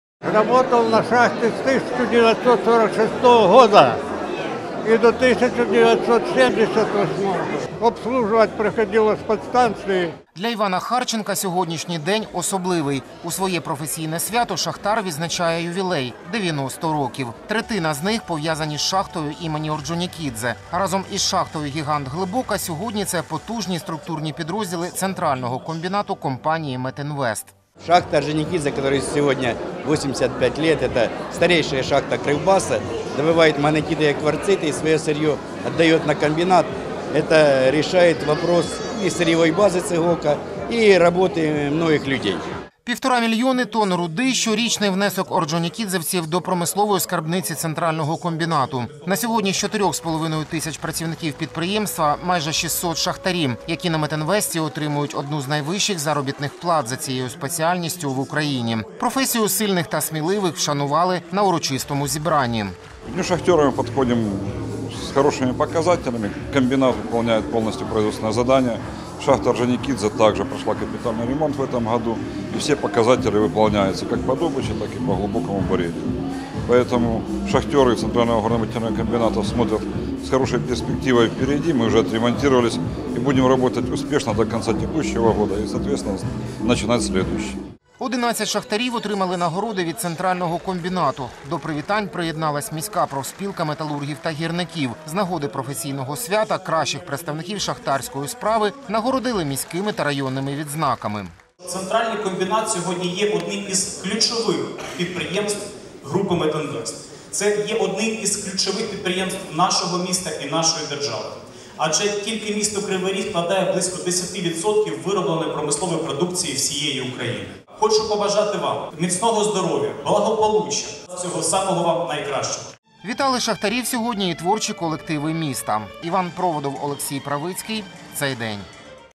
День шахтаря на Центральному комбінаті
У неділю шахтарі України відзначатимуть своє професійне свято. Особливе воно - для майже шестисот представників підземної праці на Центральному комбінаті. Шахтарів ЦГЗК вітали сьогодні у Палаці культури комбінату. З урочистостей репортаж Івана Проводова.